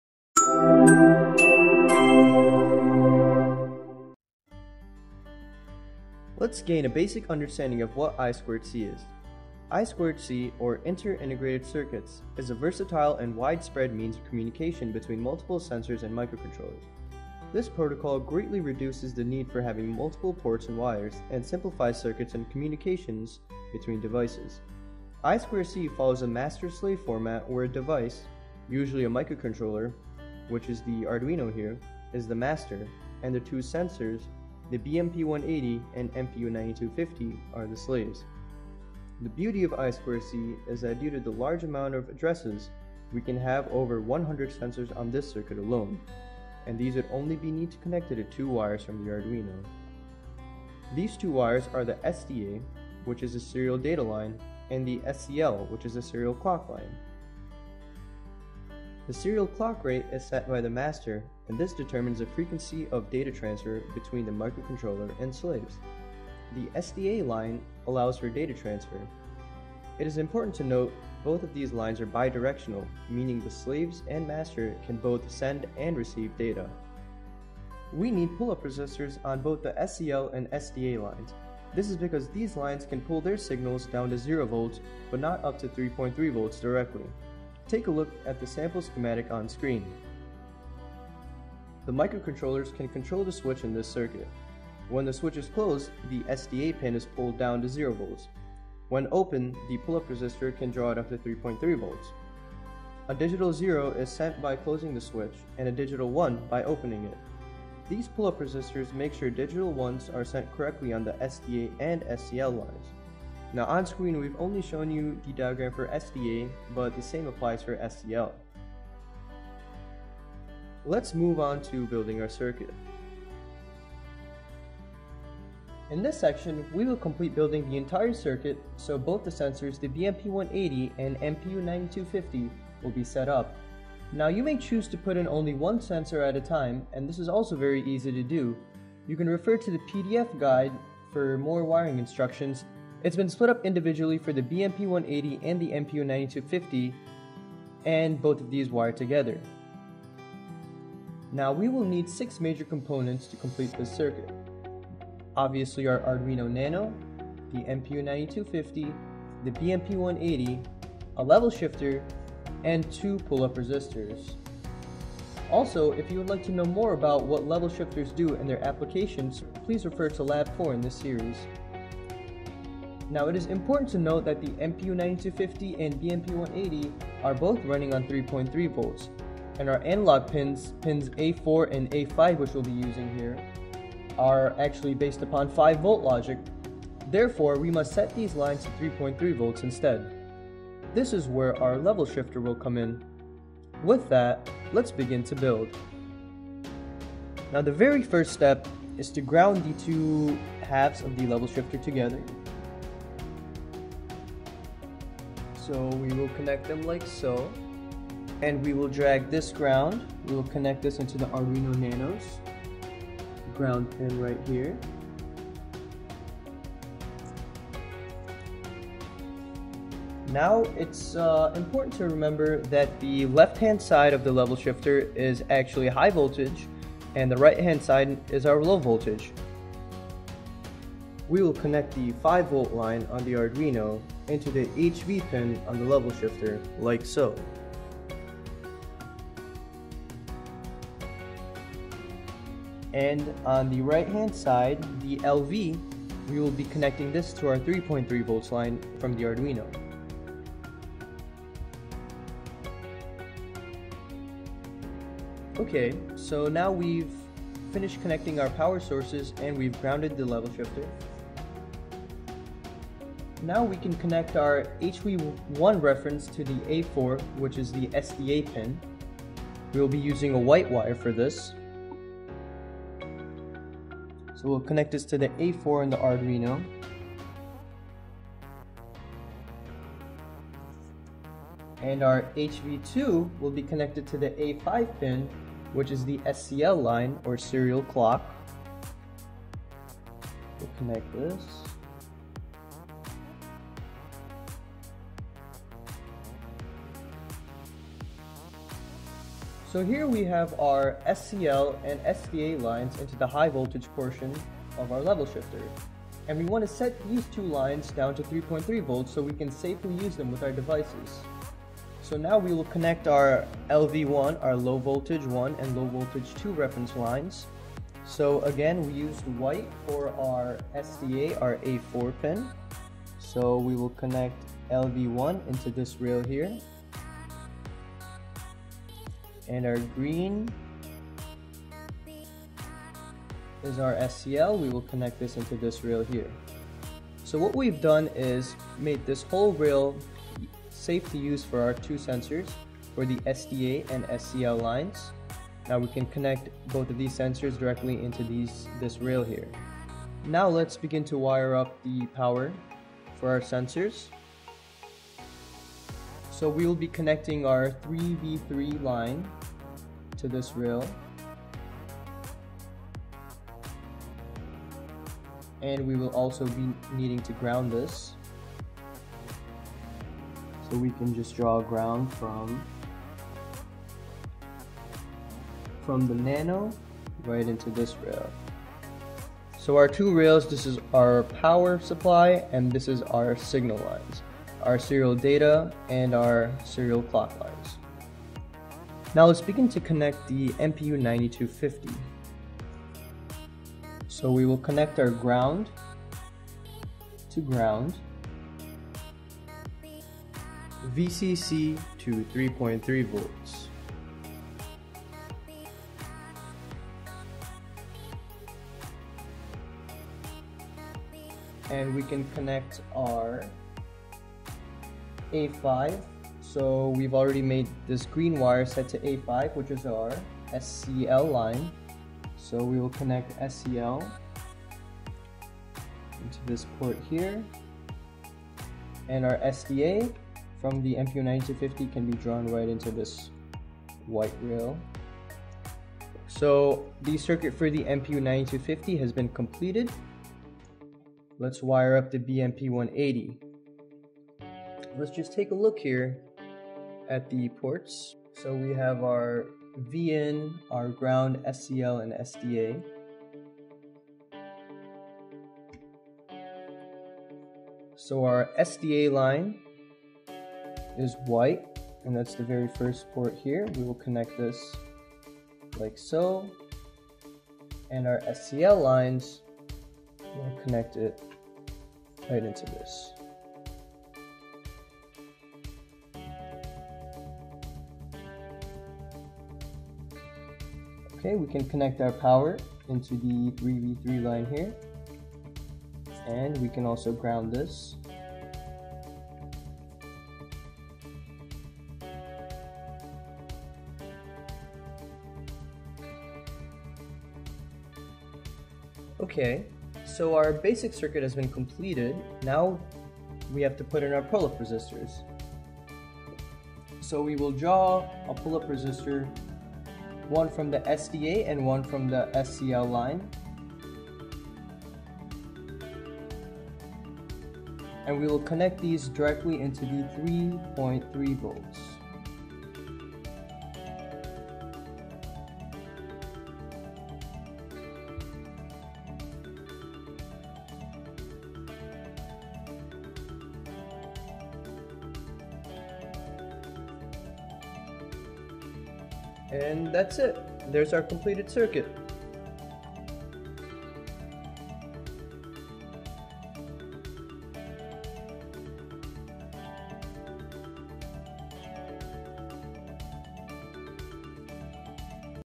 Arduino Lab 7a: I2C, Part1
Hello everyone, welcome to another Hands On Engineering tutorial. In this lab, we’ll discuss the I2C protocol. In addition, we will show how to use the MPU9250 and BMP180 sensors, both of which use I2C as their primary communications method.

This is part 1 of 3. In this part, we go over how to build the circuits that let us interface with I2C sensors.

As always, here’s the link to the pdf for this lab. Check it out if you have any questions on the material we covered in this lab. We go more deeply into many of the concepts covered in this lab in the introduction of the pdf. We’ve also included links to the datasheets and I2C scanner.

Sincerely,

The Hands on Engineering Team.